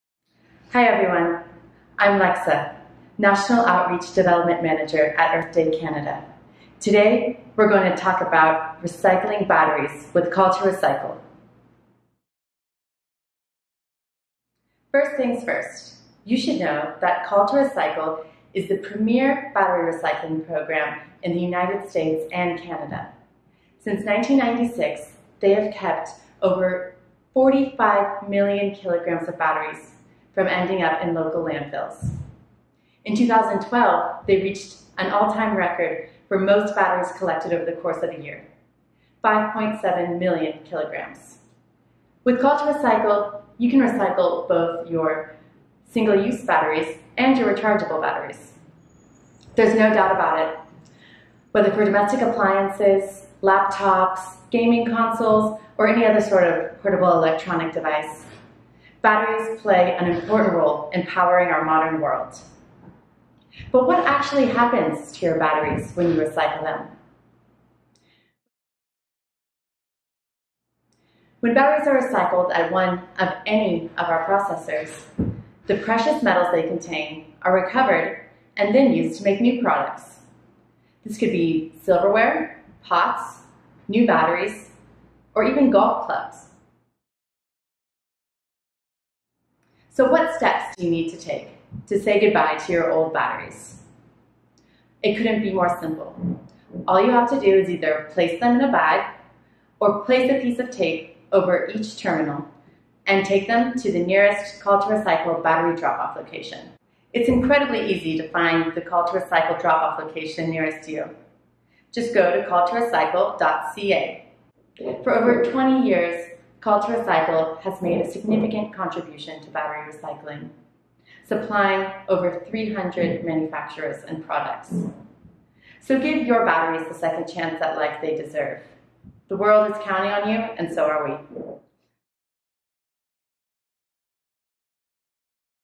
Recycle your batteries with Call2Recycle!
Batteries! They've been part of your everyday life for almost as long as you can remember.
In your remote controls, laptops, game consoles, laptops, or other portable electronic devices - they're everywhere!
But do you have the fully story on your batteries and what happens to them after recycling with Call2Recycle Canada? We’re here to give you the scoop!